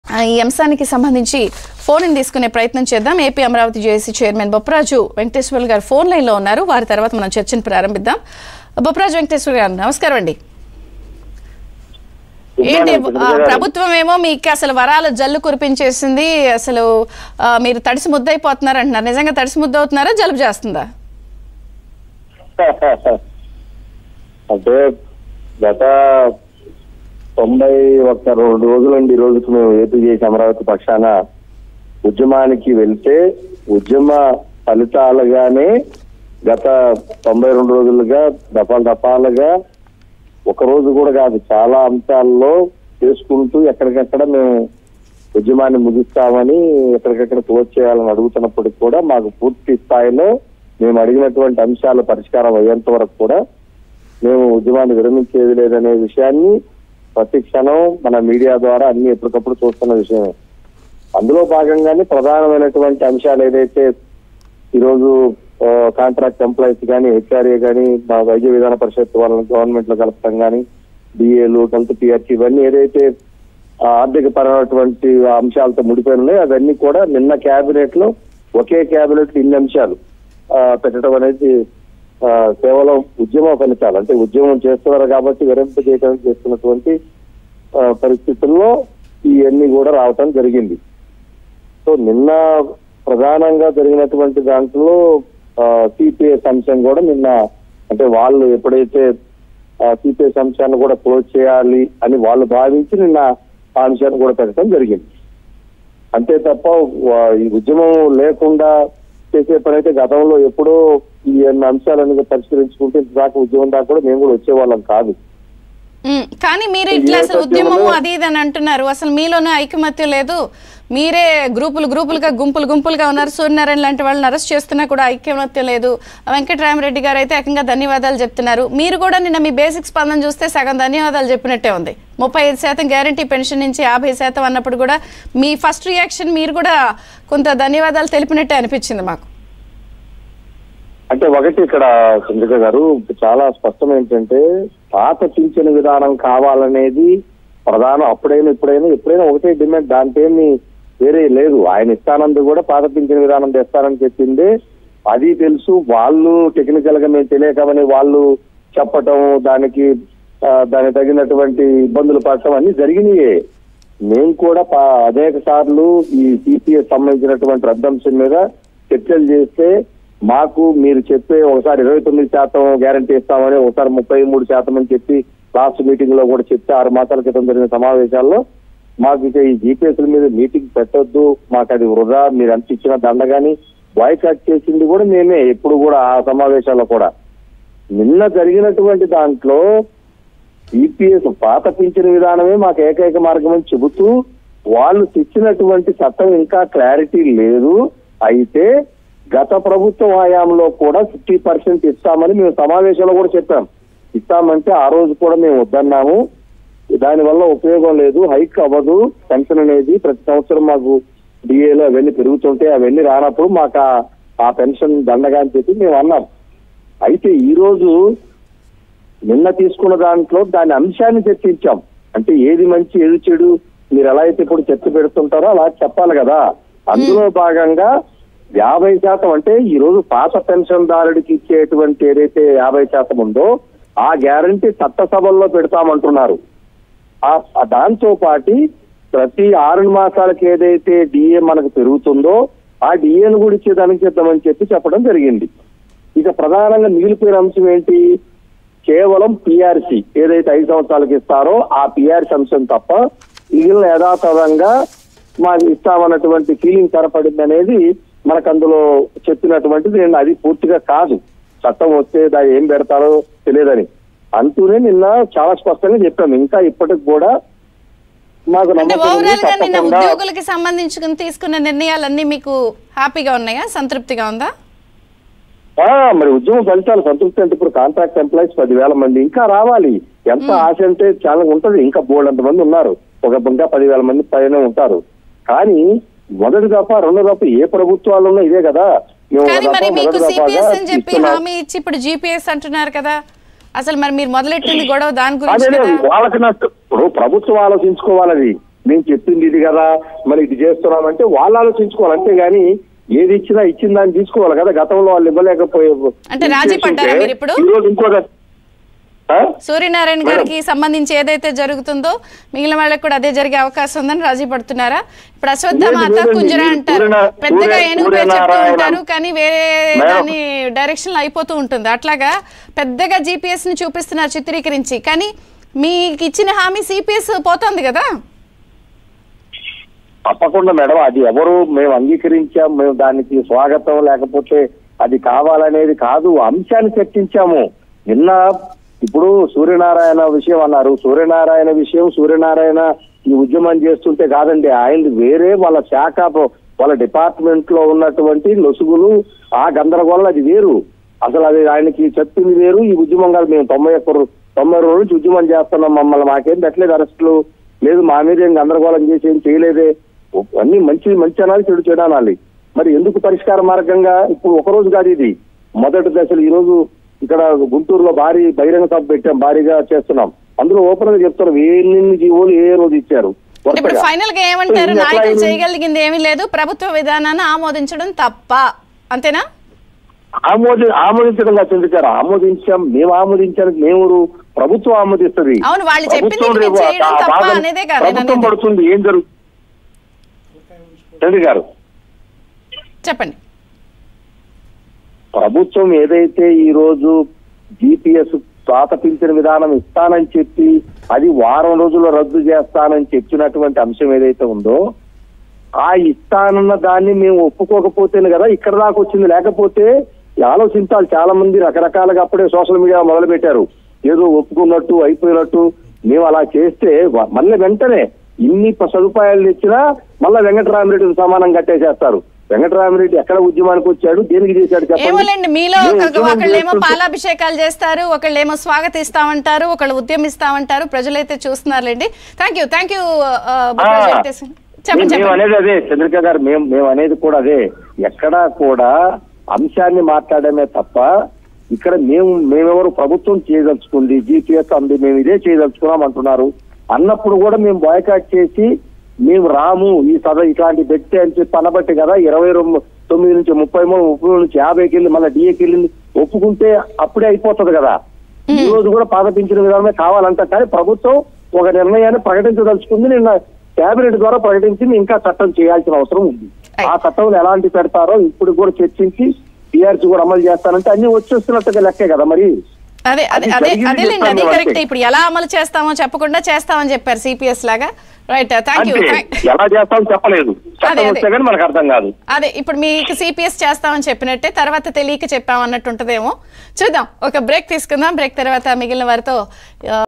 Bopparaju Venkateshwarlu : సీపీఎస్ స్థానంలో జీపీఎస్...లాభాలేంటి ? నష్టాలేంటి..? || ABN Telugu
🔴Live: ఎన్నికల ముందు ఎన్ని సిత్రాలో.. ఆఖరి ప్రయత్నం | YS Jagan | CBN | ABN @ BREAKFAST